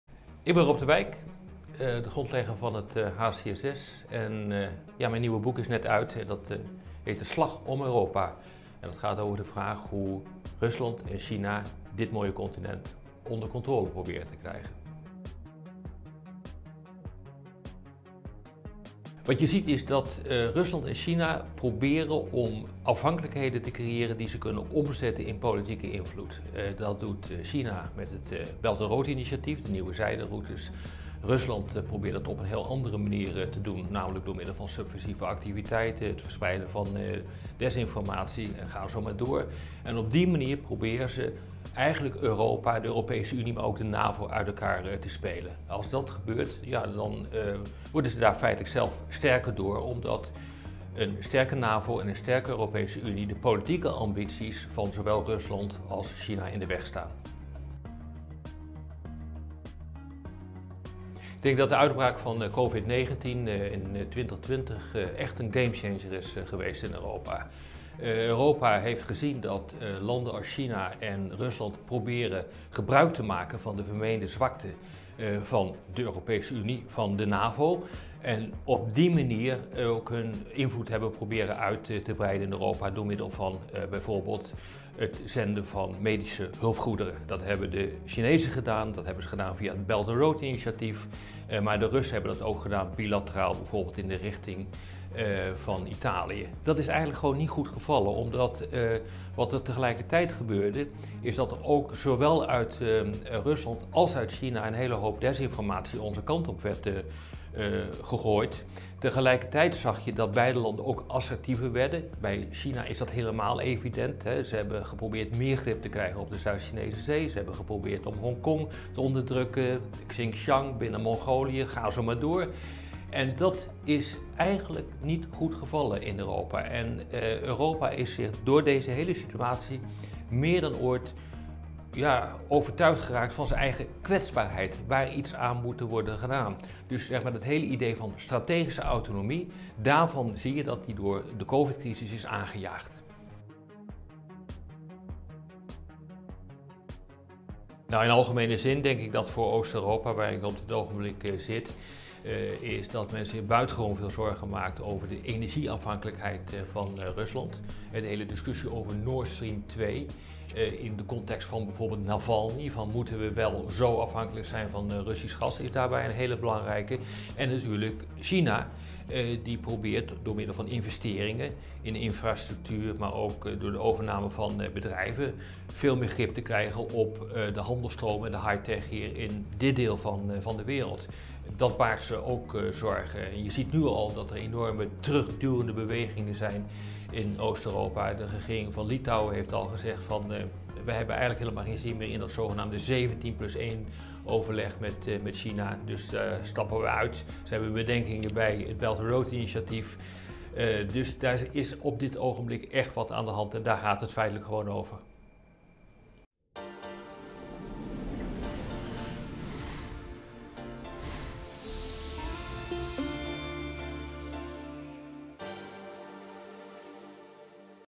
Rob de Wijk: Hebben China en Rusland 'de slag om Europa' gewonnen?
Rob de Wijk is oprichter van het Den Haag Centrum voor Strategische Studies, hoogleraar Internationale Betrekkingen aan de Universiteit Leiden, en schrijver van het nieuwe boek: "De Slag om Europa."

Deze pitch was onderdeel van het HCSS Symposium over “Engagement, Dependence, Competition and Conflict: Dutch and European relations with China and Russia in an era of ‘extreme competition’.” Lees hier meer over het evenement